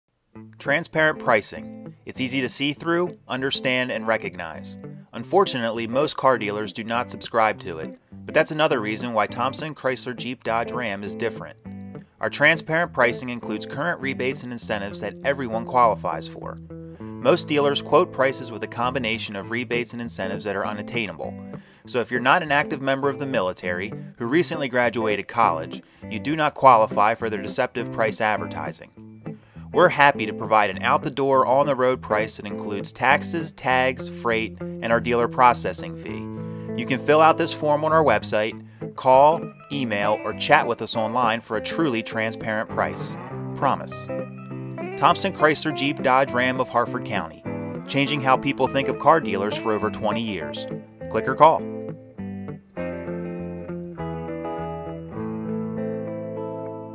Transparent Pricing
Another reason Thompson Chrysler Dodge Jeep RAM is different from most car dealers is our transparent pricing. It's easy to understand and we don't play the games that frustrate a lot of online car shoppers.